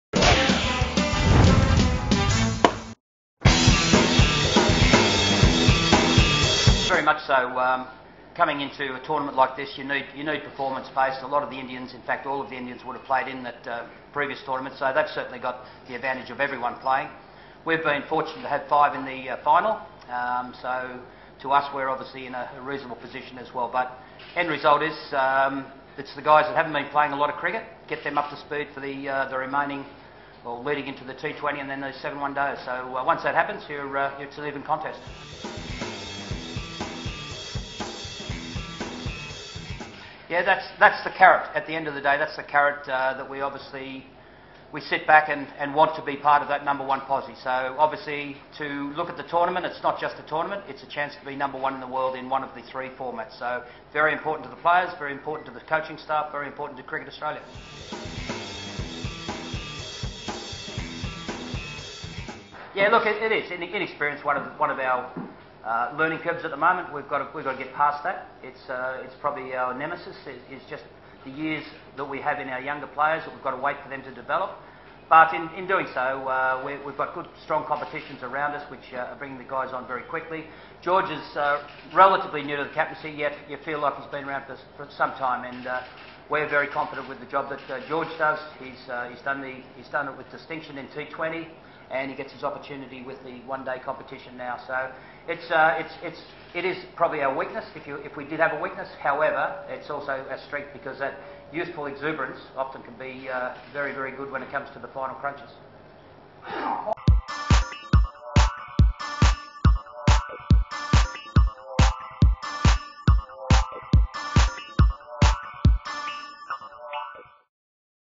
Australia aim to be No 1 ODI side: Steve Rixon
Their focus on the number one spot up for grabs in the upcoming One-Day International (ODI) series against India, Australian captain George Bailey and coach Steve Rixon said the visitors are no longer intimidated by the sub-continental conditions courtesy the exposure they have got through the Indian Premier League (IPL).

The Aussies will start the tour with a one-off Twenty20 International in Rajkot on October 10 followed by seven ODIs.

(with inputs from PTI)